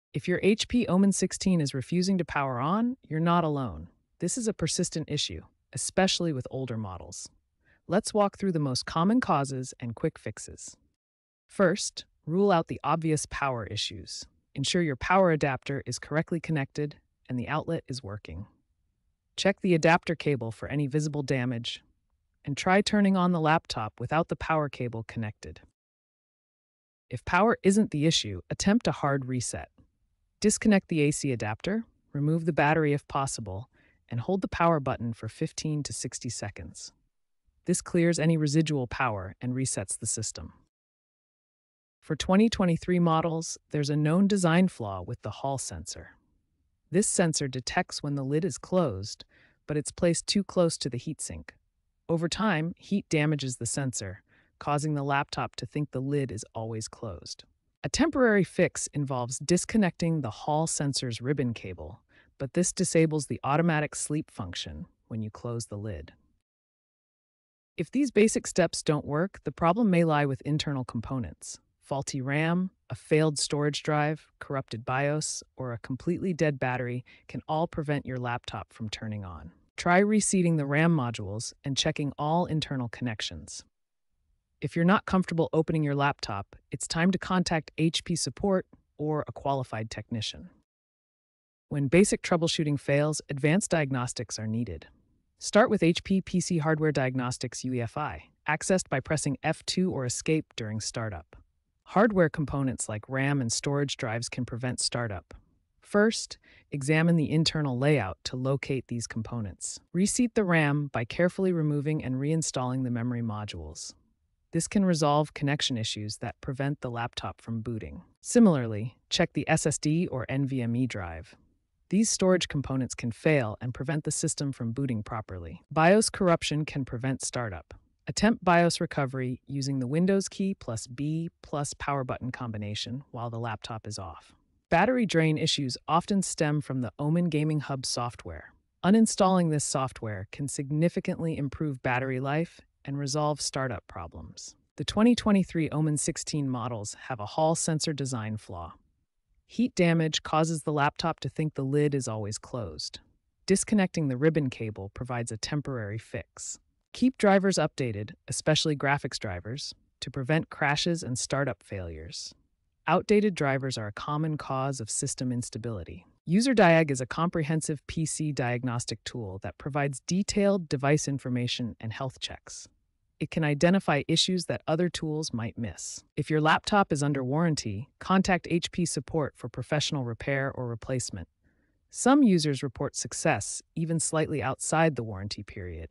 HP Omen 16 Not Turning On – Power Troubleshooting & Fix Guide (2026)
In this comprehensive guide, we tackle the frustrating issue of the HP Omen 16 not turning on. If you're experiencing power troubles with your gaming laptop, this video provides step-by-step troubleshooting techniques and effective fixes to get your device back up and running. We cover common causes of power failure, including battery issues, power supply problems, and hardware malfunctions.